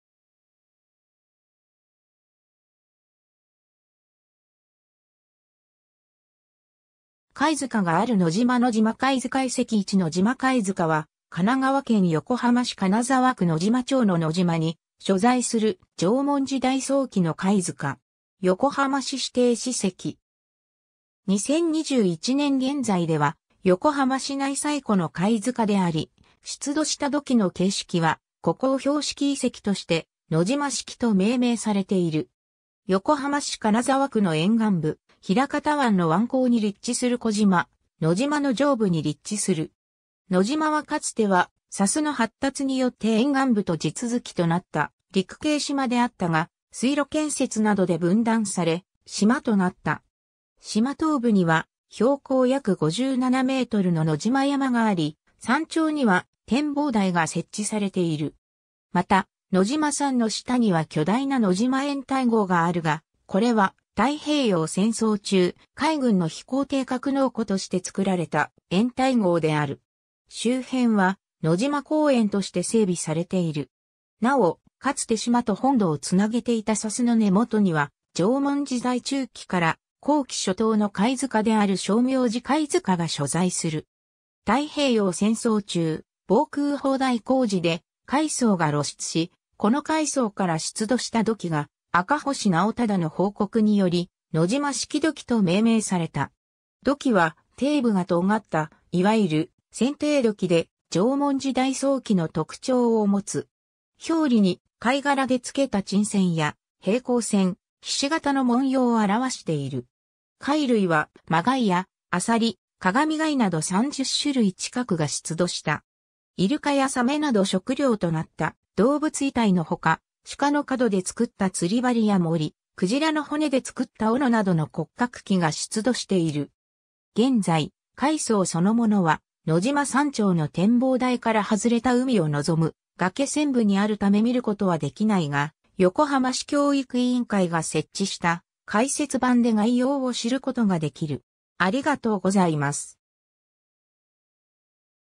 野島貝塚
貝塚がある野島(2007年9月17日撮影) 野島貝塚 遺跡位置 野島貝塚（のじまかいづか）は、神奈川県横浜市金沢区野島町の野島に所在する縄文時代早期（およそ8000年前）の貝塚。
横浜市指定史跡。
2021年（令和3年）現在では横浜市内最古の貝塚であり、出土した土器の型式は、ここを標式遺跡として「野島式」と命名されている。
 横浜市金沢区の沿岸部、平潟湾の湾口に立地する小島「野島」の上部に立地する。
 野島はかつては砂州の発達によって沿岸部と地続きとなった陸繋島であったが、水路建設などで分断され、島となった。
島東部には標高約57メートルの野島山があり、山頂には展望台が設置されている。
また、野島山の下には巨大な野島掩体壕があるが、これは太平洋戦争中、海軍の飛行艇格納庫として作られた掩体壕である（実際は使用されずに終戦を迎えた。また、現在は封鎖されて立入りできない）。
周辺は野島公園として整備されている。
 なお、かつて島と本土を繋げていた砂州の根元（国の史跡・称名寺が所在する寺前・金沢町地区）には、縄文時代中期～後期初頭の貝塚である称名寺貝塚が所在する。
 太平洋戦争中、防空砲台工事で貝層が露出し、この貝層から出土した土器が、赤星直忠の報告により「野島式土器」と命名された。
土器は底部が尖ったいわゆる「尖底土器」で、縄文時代早期の特徴を持つ。
表裏に貝殻でつけた沈線や平行線・菱形の文様を表している。
貝類は、マガイやアサリ、カガミガイなど30種類近くが出土した。
イルカやサメなど食料となった動物遺体のほか、シカの角で作った釣針や銛、クジラの骨で作った斧などの骨角器が出土している。
 現在、貝層そのものは野島山頂の展望台から外れた海を望む崖線部にあるため見ることはできないが、横浜市教育委員会が設置した解説板で概要を知ることができる。